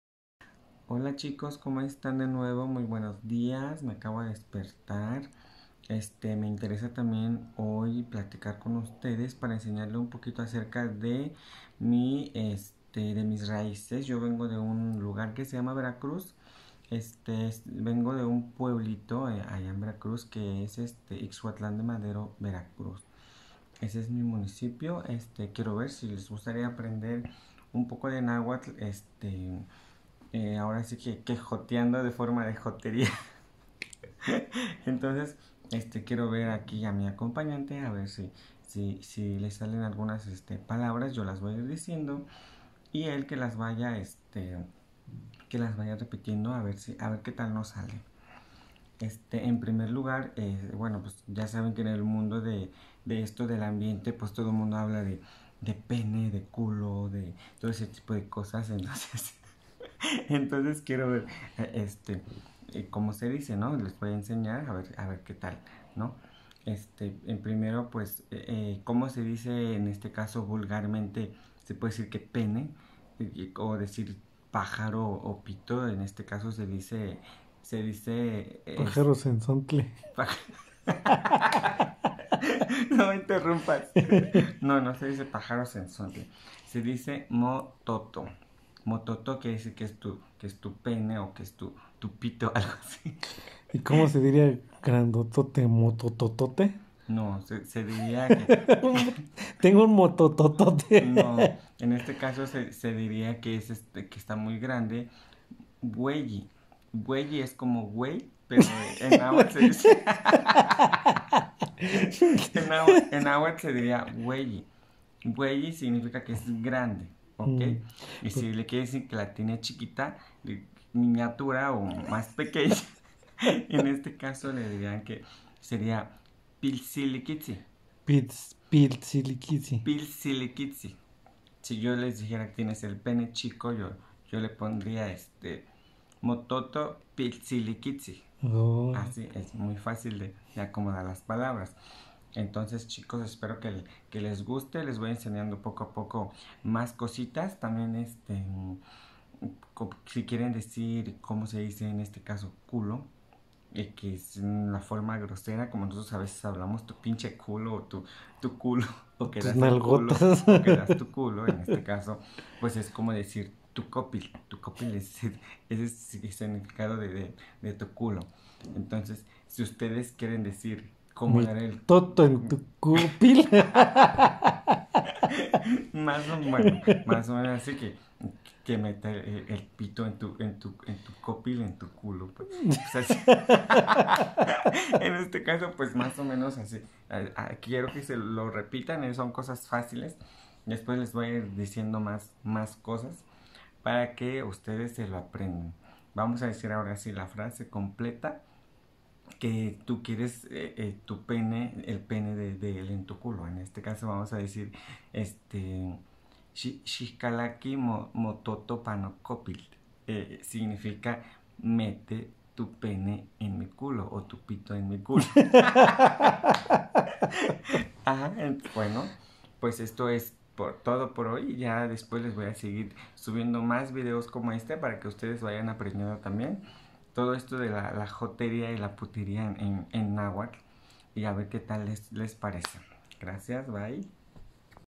joterias en nahuatl
Como decir pene y culo en nahuatl